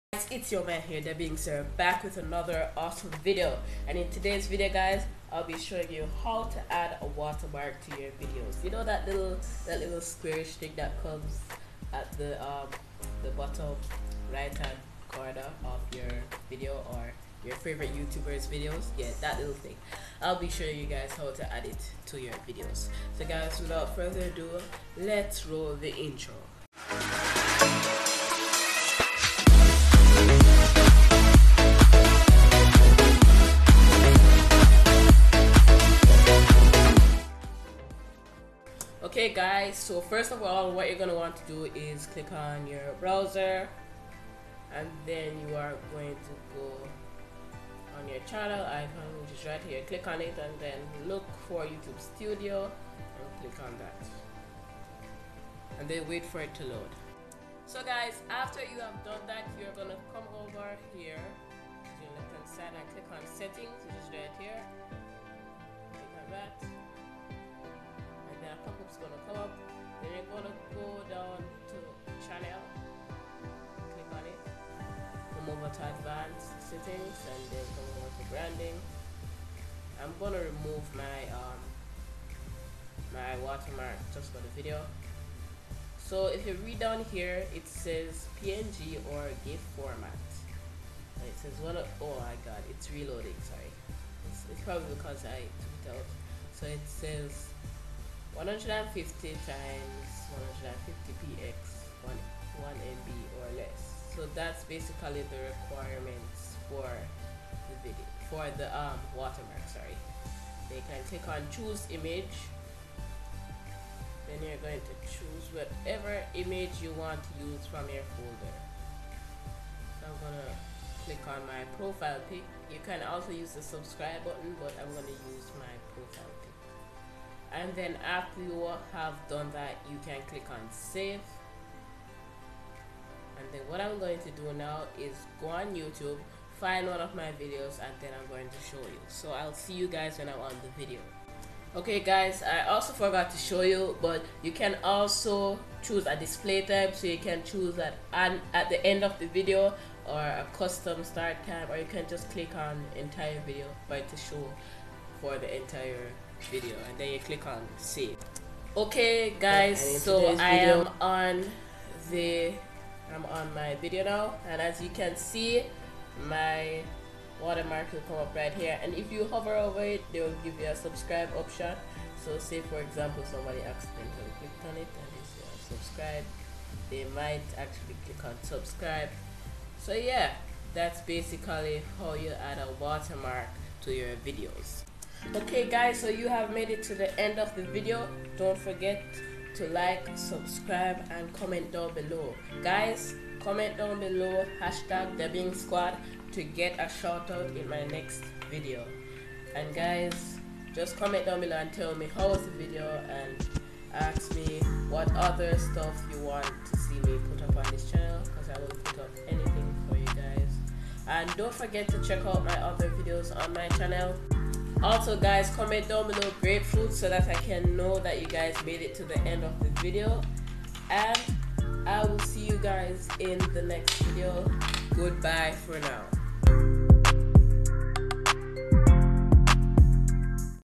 HOW TO ADD A WATERMARK TO YOUR VIDEOS 2020!!!!
HEY GUYS IN THIS VIDEO IM GONNA SHOW YOU GUYS HOW TO ADD A WATERMARK TO YOUR VIDEOS DONT FORGET TO LIKE SUBSCRIBE AND ILL SEE YALL IN THE NEXT VIDEO